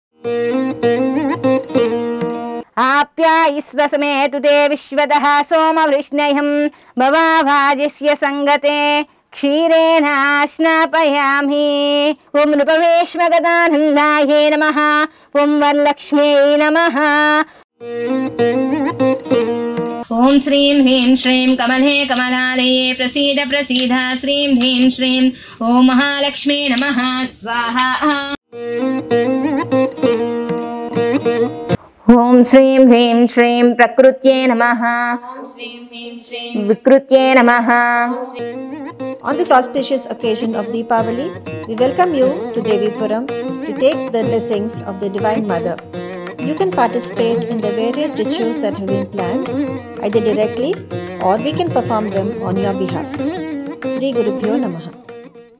Deepavali@Devipuram
Worship Sri Mahalakshmi on the auspicious occasion of Deepavali